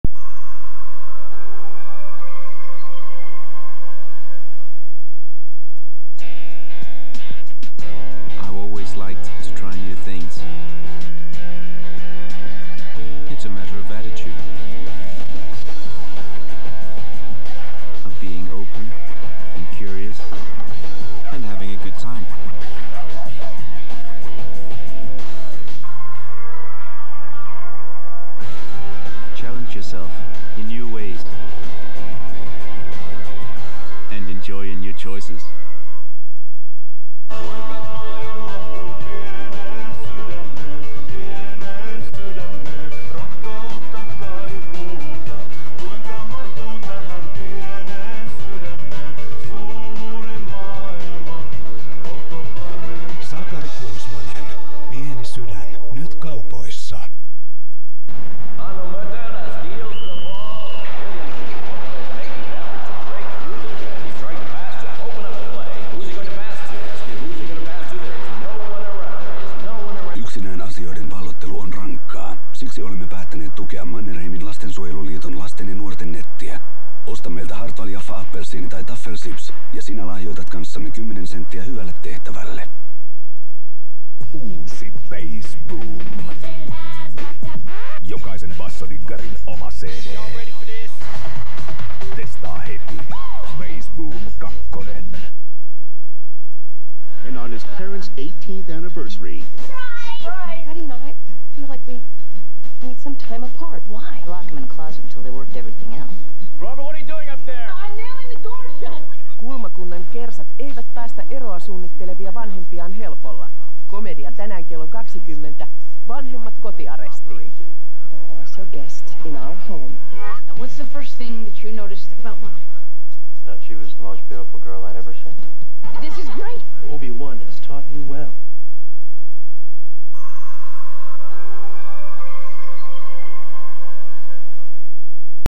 MTV3 heinäkuu 2002? - Mainoskatko #2
Ohjelman Vintiöt aikaiset mainokset.